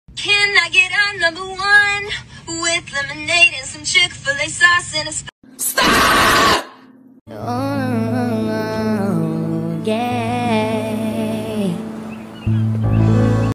stan twitter: girl sings her chic-fil-a order
entire video edited by me.